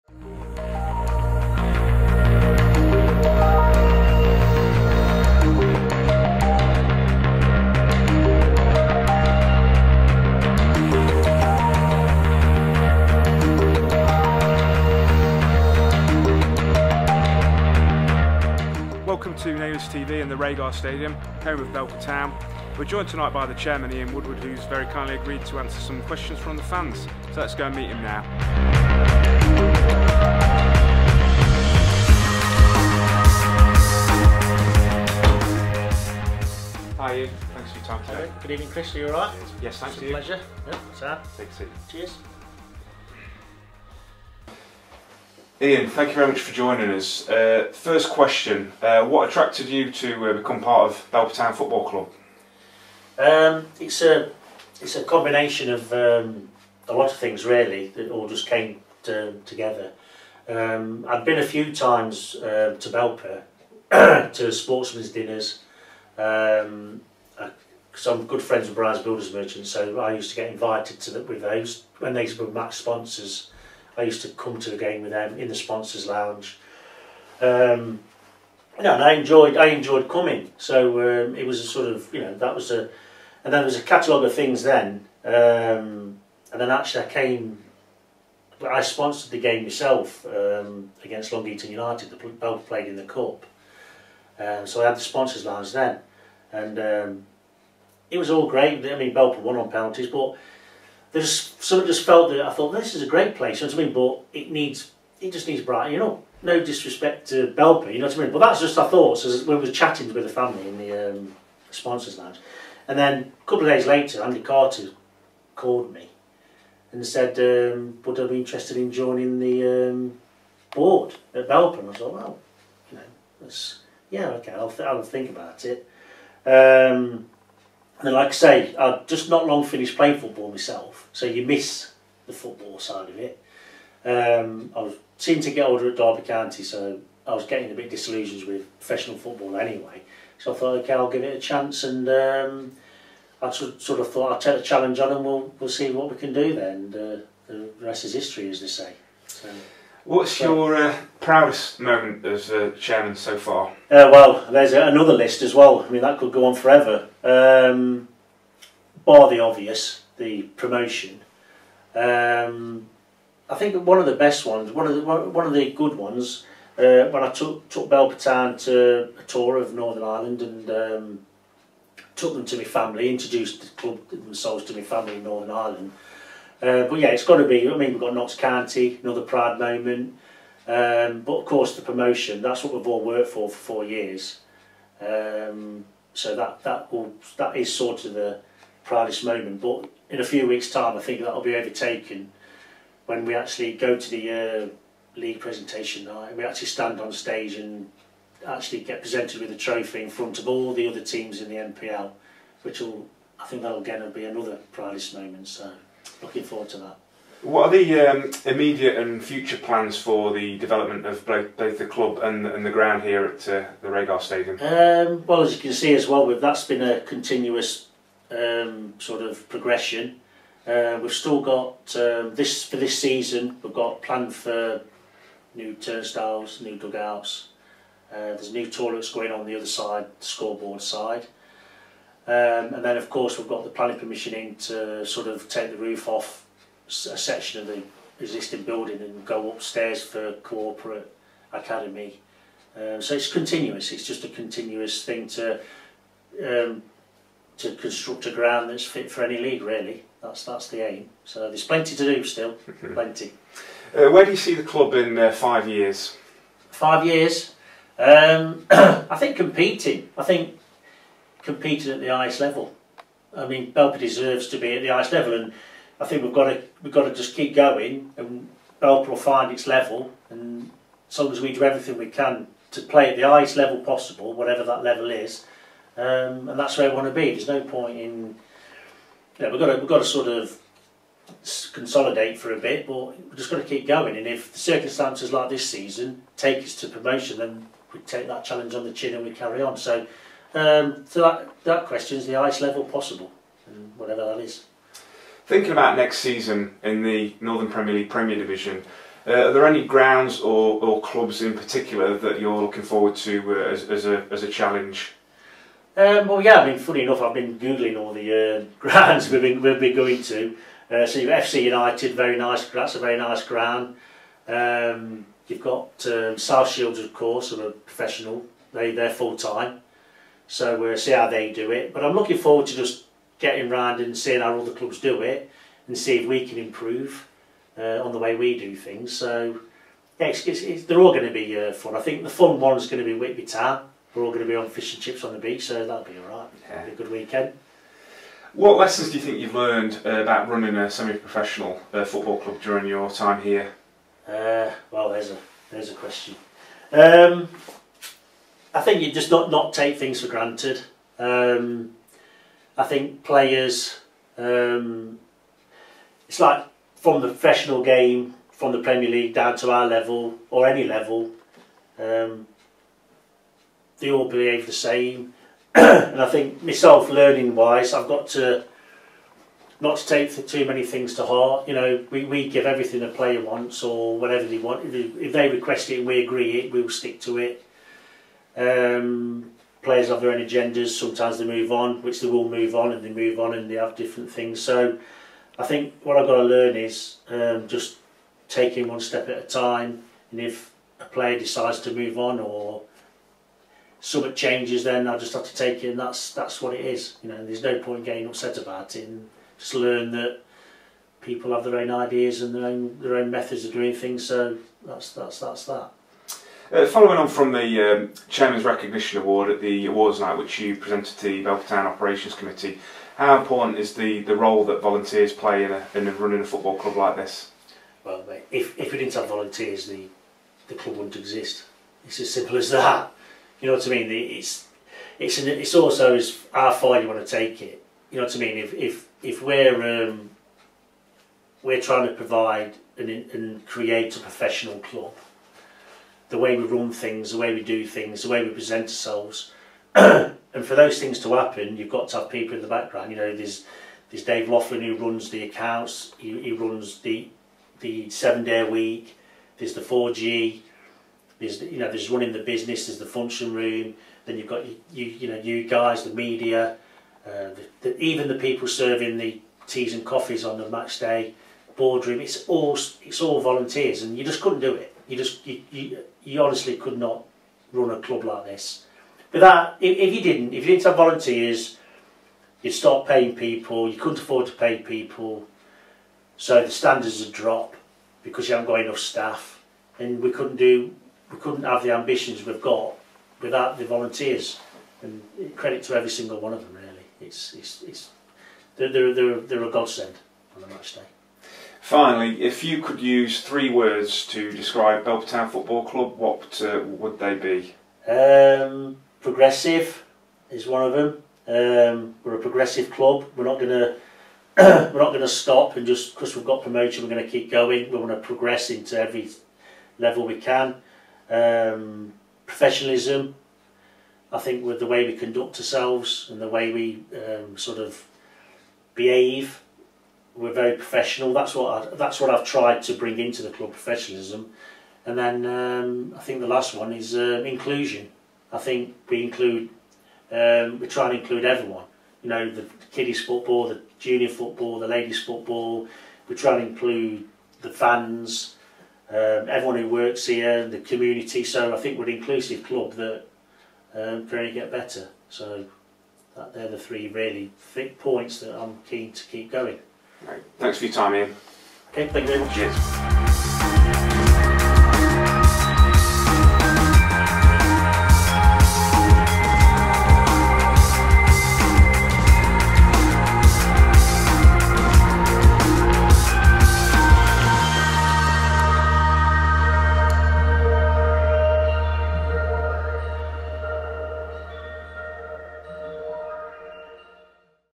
Sit down with the Chairman - Ian Woodward
In collaboration with Take2.org, we filmed with the chairman of Belper Town to understand why he chose Belper, how he got involved and plans for the future.

This video was filmed by - Mark Ashwell & Seb Mannell
Interviewer - Chris Nelson